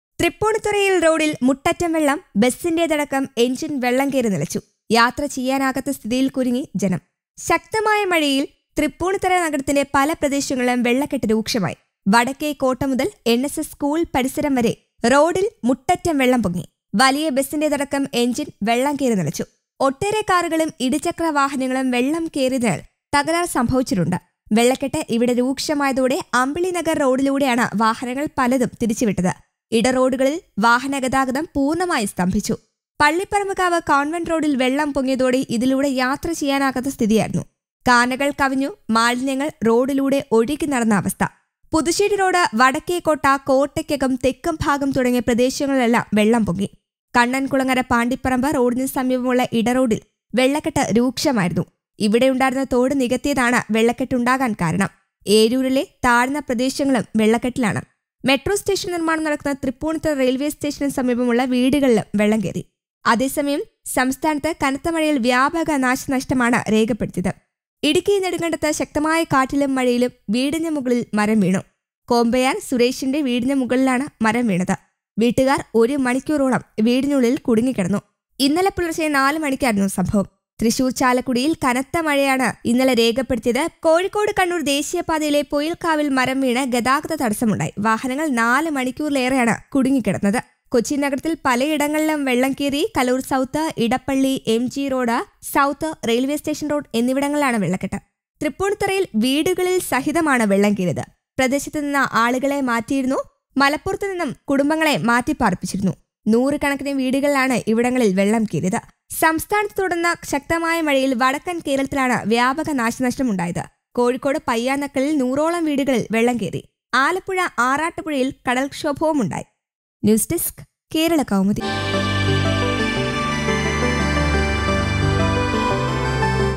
തൃപ്പുണിത്തുറ മുങ്ങി,റോഡില്‍ മുട്ടറ്റം വെള്ളം.ദുരിതത്തില്‍ ജനം | Thripunithura | Kerala Rains
.തൃപ്പൂണിത്തുറയില്‍ റോഡില്‍ മുട്ടറ്റം വെള്ളം; ബസിന്റെതടക്കം എന്‍ജിന്‍ വെള്ളം കയറി നിലച്ചു, യാത്ര ചെയ്യാനാകാത്ത സ്ഥിതിയില്‍ കുരുങ്ങി ജനം..ശക്തമായ മഴയില്‍ നഗരത്തിന്റെ പല പ്രദേശങ്ങളിലും വെള്ളക്കെട്ടു രൂക്ഷമായി. വടക്കേക്കോട്ട മുതല്‍ എന്‍എസ്എസ് സ്‌കൂള്‍ പരിസരം വരെ റോഡില്‍ മുട്ടറ്റം വെള്ളം പൊങ്ങി.വലിയ ബസിന്റെതടക്കം എന്‍ജിന്‍ വെള്ളം കയറി നിലച്ചു. ഒട്ടേറെ കാറുകളും ഇരുചക്ര വാഹനങ്ങളും വെള്ളം കയറിയതിനാല്‍ തകരാര്‍ സംഭവിച്ചിട്ടുണ്ട്.വെള്ളക്കെട്ട് ഇവിടെ രൂക്ഷമായതോടെ അമ്ബിളി നഗര്‍ റോഡിലൂടെയാണ് വാഹനങ്ങള്‍ പലതും തിരിച്ചു വിട്ടത്. ഇട റോഡുകളില്‍ വാഹനഗതാഗതം പൂര്‍ണമായും സ്തംഭിച്ചു. പള്ളിപ്പറമ്ബ്കാവ്  കോണ്‍വന്റ് റോഡില്‍ വെള്ളം പൊങ്ങിയതോടെ ഇതിലൂടെ യാത്ര ചെയ്യാനാകാത്ത സ്ഥിതിയായിരുന്നു. കാനകള്‍ കവിഞ്ഞു മാലിന്യങ്ങള്‍ റോഡിലൂടെ ഒഴുകി നടന്ന അവസ്ഥ.പുതുശേരി റോഡ്, വടക്കേക്കോട്ട, കോട്ടയ്ക്കകം, തെക്കുംഭാഗം തുടങ്ങിയ പ്രദേശങ്ങളിലെല്ലാം വെള്ളം പൊങ്ങി. കണ്ണന്‍കുളങ്ങര പാണ്ടിപ്പറമ്ബ് റോഡിനു സമീപമുള്ള ഇടറോഡില്‍ വെള്ളക്കെട്ടു രൂക്ഷമായിരുന്നു. ഇവിടെയുണ്ടായിരുന്ന തോട് നികത്തിയതാണു വെള്ളക്കെട്ടു ഉണ്ടാകാന്‍ കാരണം. എരൂരിലെ താഴ്ന്ന പ്രദേശങ്ങളും വെള്ളക്കെട്ടിലാണ്. മെട്രോ സ്റ്റേഷന്‍ നിര്‍മാണം നടക്കുന്ന തൃപ്പൂണിത്തുറ റെയില്‍വേ സ്റ്റേഷനു സമീപമുള്ള വീടുകളിലും വെള്ളം കയറി.